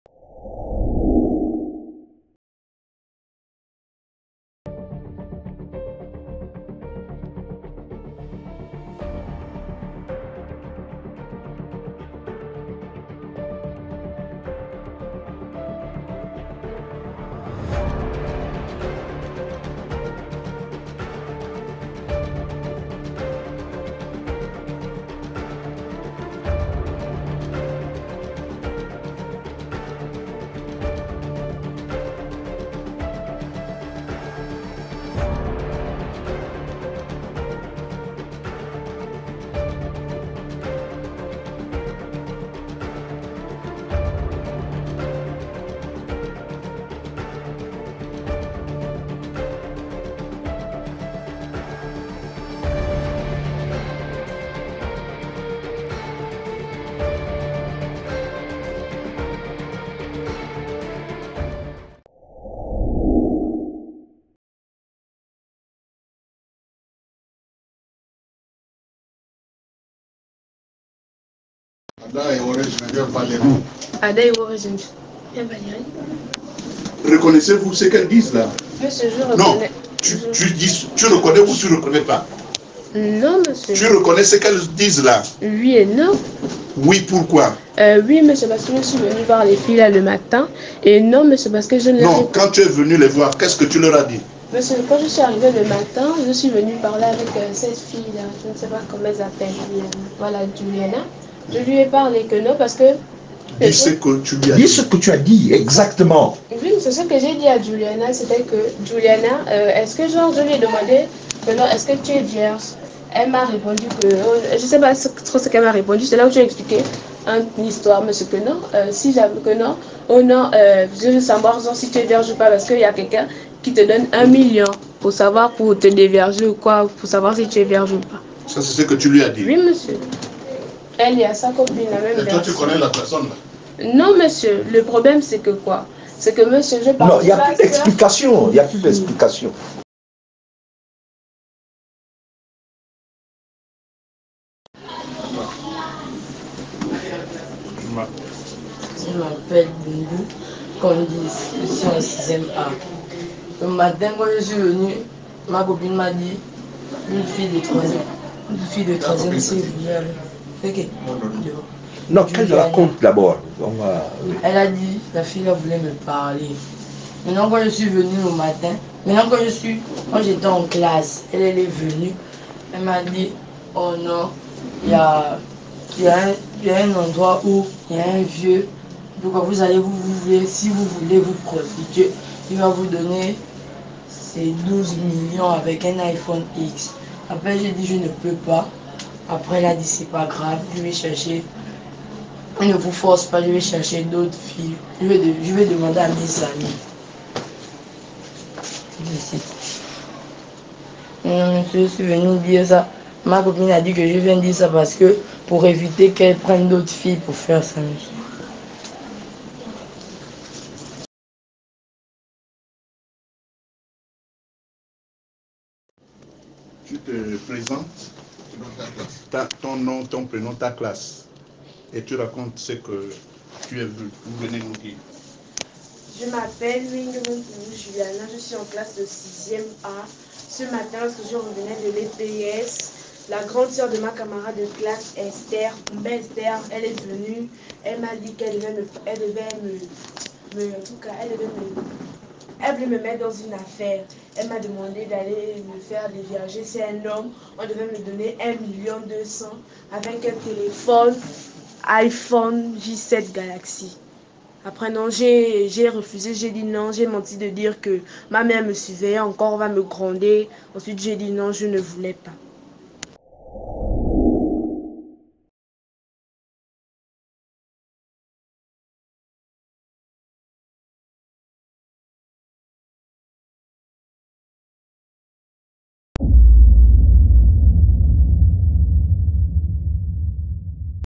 Gabon: Prostitution au Lycée Sainte-Marie.
Gabon: Un réseau de prostitution en milieu scolaire dans la capitale Librevilloise. Une adolescente de 16 ans propose à une élève de 10 ans en classe de 6ème de se faire dépuceler par un monsieur âgé de 65 ans pour une somme de 1 million et un smartphone...